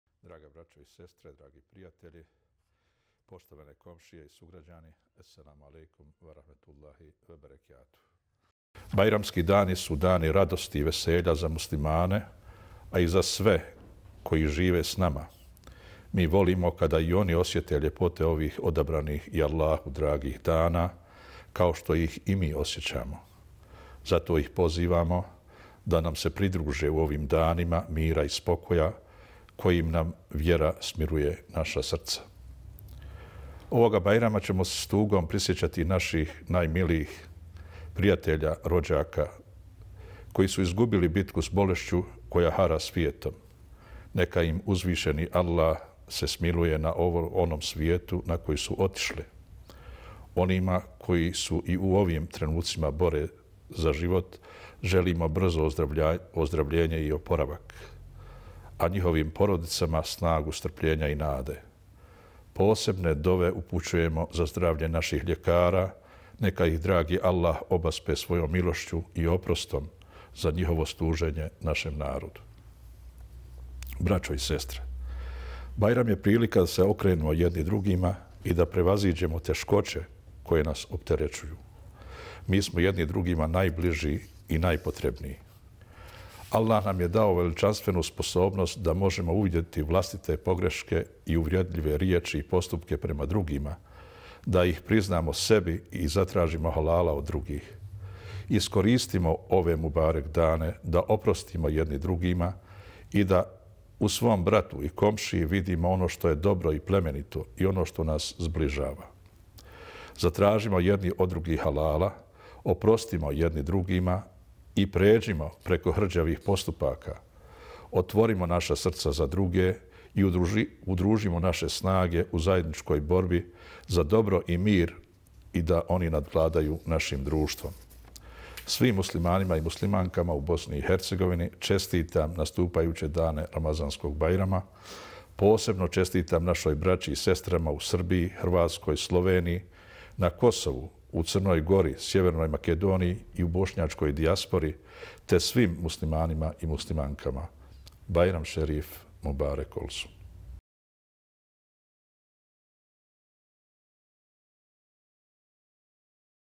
Kavazović: Udružimo se da dobro i mir nadvladaju našim društvom
SARAJEVO, 12. maja (FENA) - Reisu-l-ulema Islamske zajednice (IZ) u Bosni i Hercegovini Husein ef. Kavazović povodom Ramazanskog bajrama uputio je danas bajramsku poruku muslimanima u Bosni i Hercegovini, domovinskim zemljama i dijaspori.